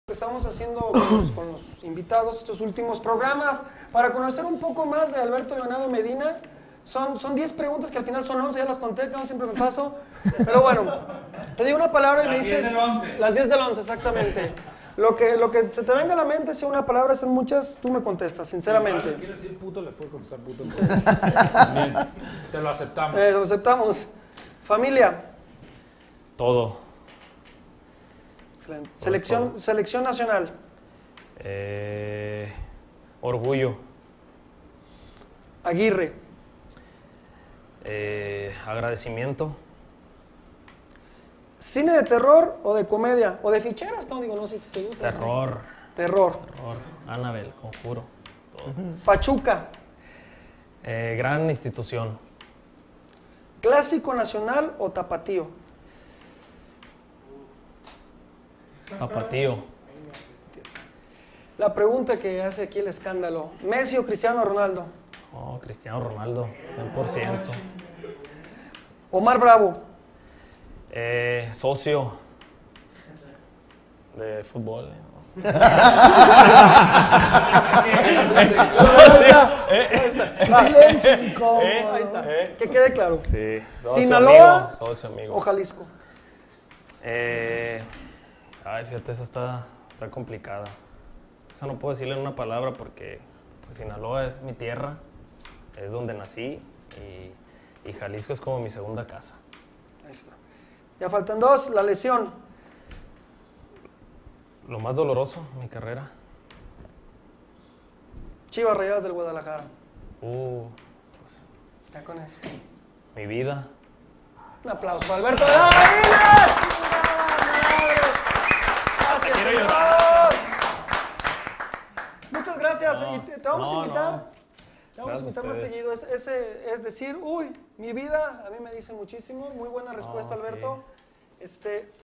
Al Chile con el Wero: las 10 rápidas del Venado Medina
Alberto "Venado" Medina platicó las 10 rápidas con Wero Vargas